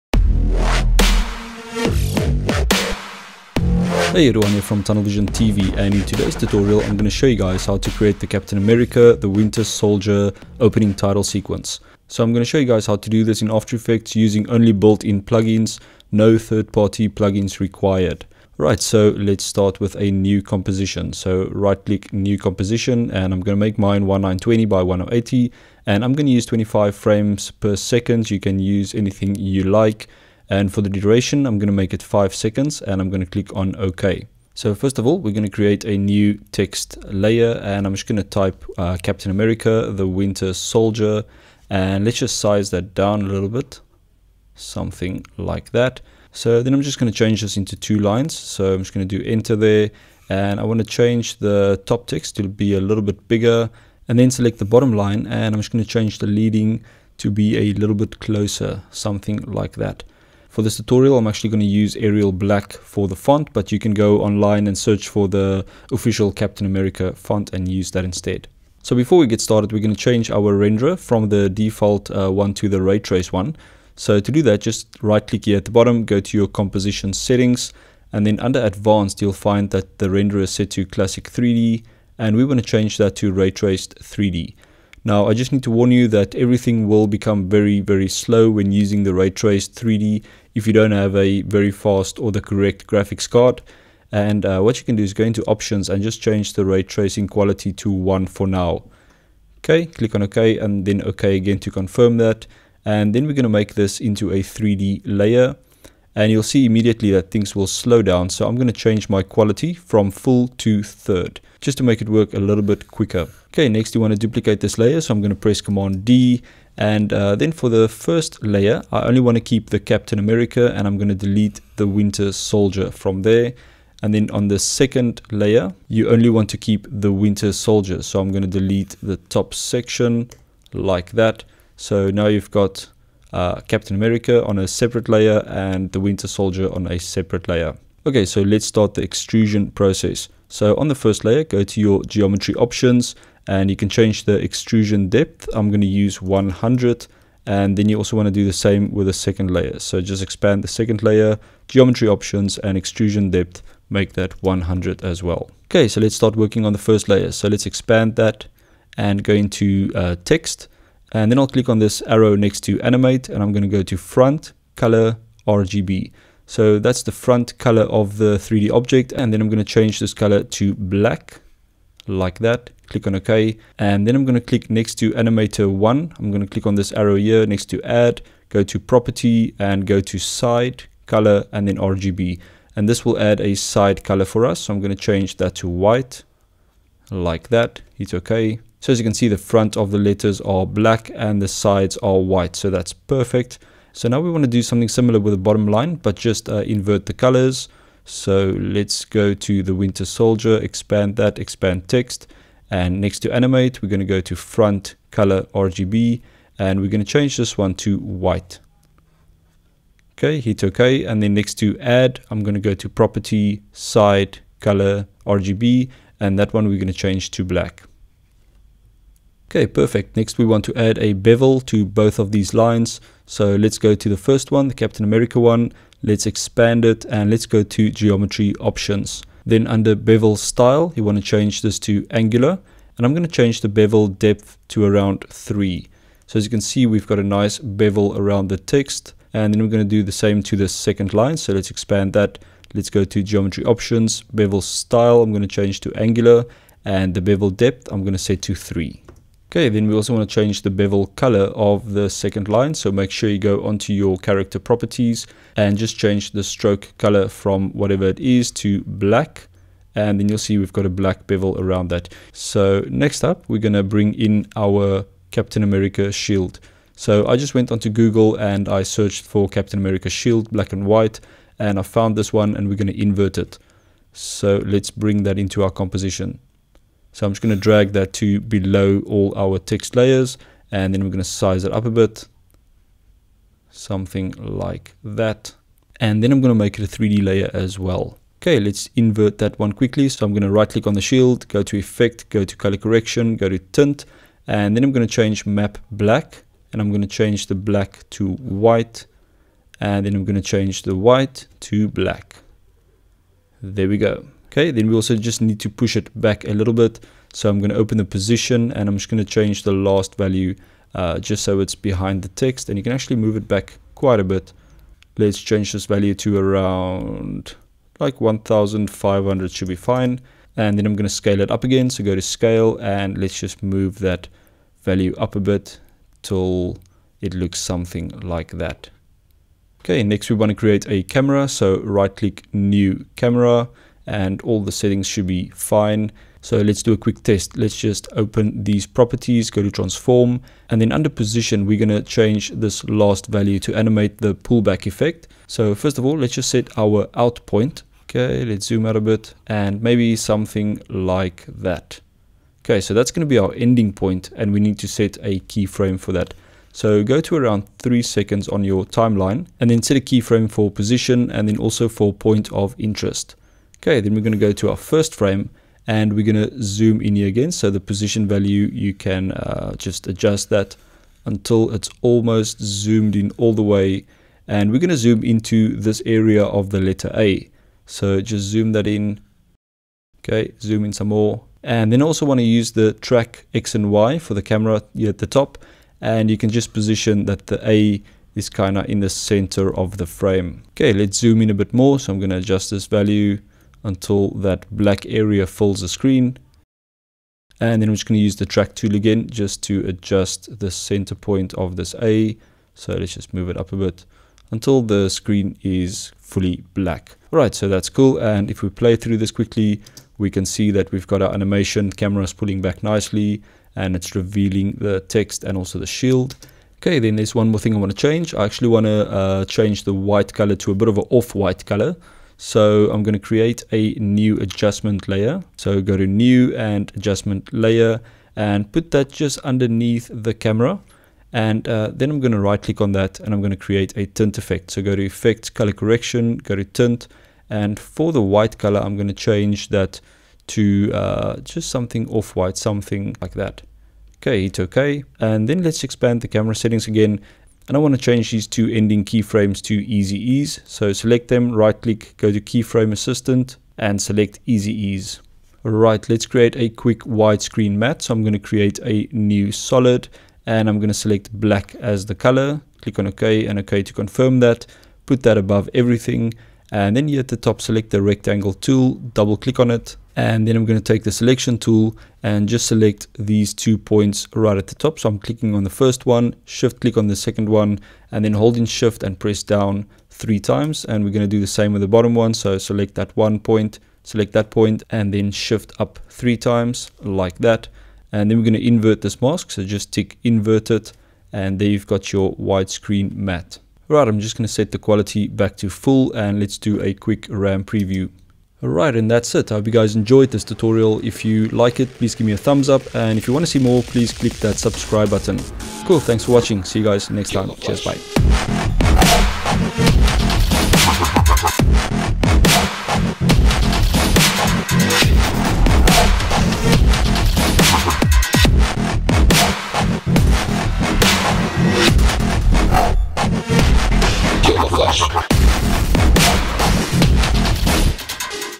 After Effects Tutorial - Captain America Title Animation
After Effects Tutorial - Captain America Title Animation - In this Adobe After Effects tutorial I show you how to re-create the opening titles for Captain America The Winter Soldier using builtin After Effects plugins.

Ruan
TunnelvizionTV